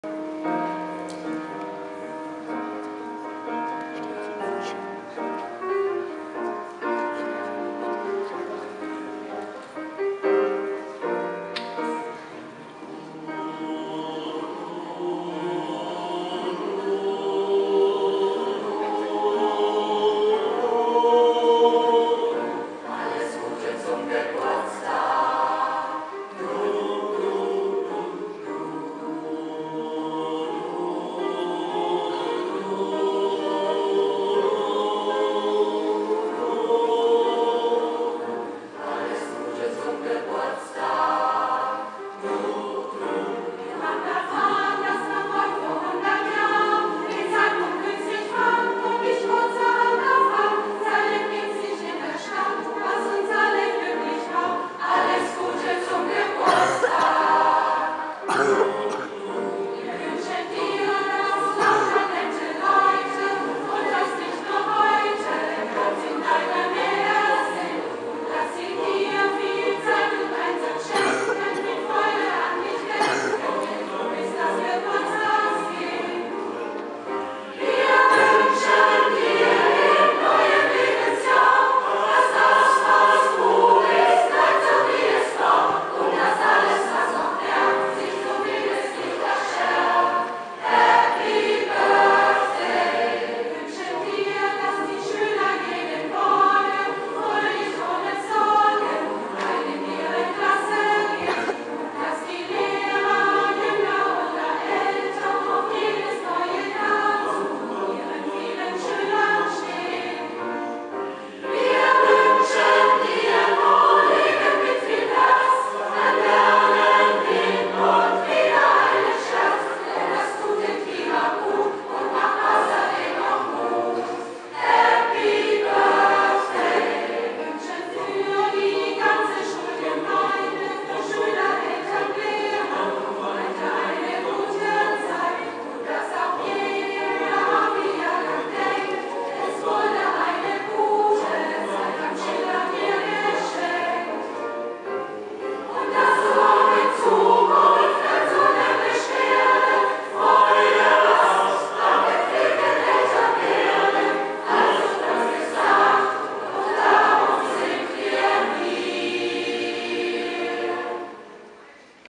Elternchor des Schiller-Gymnasiums Offenburg zum 100jährigen Schuljubiläum
Auftritt des Elternchors zur Hundertjahrfeier des Schillergymnasiums